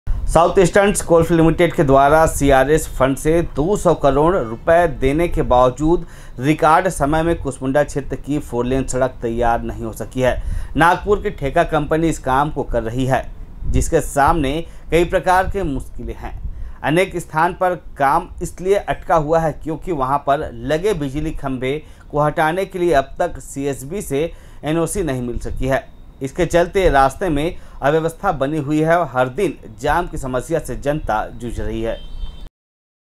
खम्भों को हटाने की एनओसी नहीं मिलने से अटका फोरलेन का काम,आए दिन जाम लगने से परेशान हो रही जनता
KORBA : खम्भों को हटाने की एनओसी नहीं मिलने से अटका फोरलेन का काम,आए दिन जाम लगने से परेशान हो रही जनता

साउथ ईस्टर्न कोलफील्ड्स लिमिटेड के द्वारा सीएसआर फंड से 200 करोड रुपए देने के बावजूद रिकॉर्ड समय में कुसमुंडा क्षेत्र की फोरलेन सड़क तैयार नहीं हो सकी है। नागपुर की ठेका कंपनी इस काम को कर रही है जिसके सामने कई प्रकार के मुश्किल है। अनेक स्थान पर काम इसलिए अटका हुआ है क्योंकि वहां पर लगे बिजली खंबे को हटाने के लिए अब तक सीएसईबी से एनओसी नहीं मिल सकी है। इसके चलते रास्ते में अव्यवस्था बनी हुई है और हर दिन जाम की समस्या से जनता जूझ रही है।